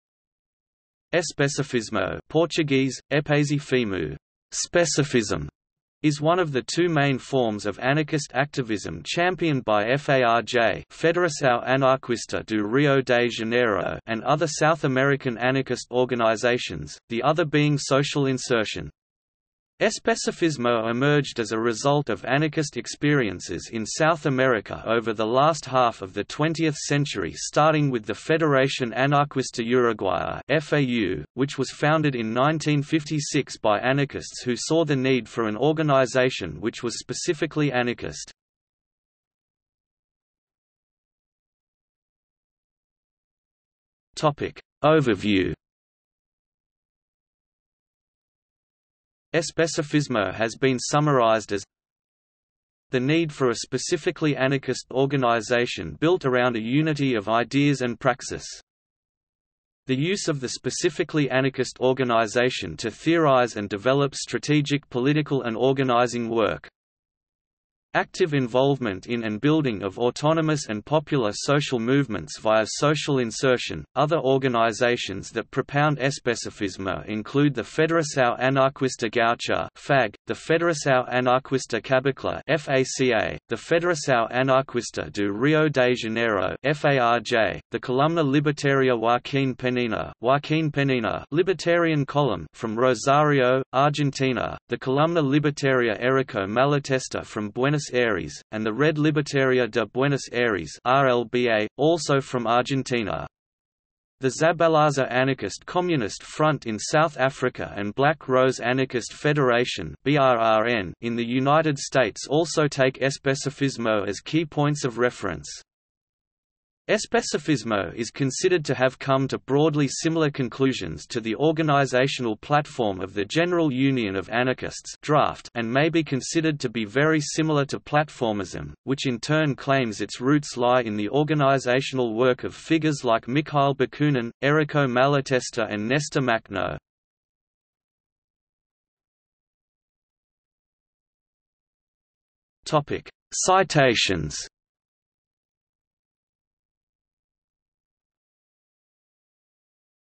Especifismo | Wikipedia audio article
00:00:40 Overview
00:02:18 Citations

- Socrates

SUMMARY
Especifismo (Portuguese: [eʃpesiˈfiʒmu], "specifism") is one of the two main forms of anarchist activism championed by FARJ (Federação Anarquista do Rio de Janeiro) and other South American anarchist organizations, the other being social insertion. Especifismo emerged as a result of anarchist experiences in South America over the last half of the 20th century starting with the Federación Anarquista Uruguaya (FAU), which was founded in 1956 by anarchists who saw the need for an organization which was specifically anarchist.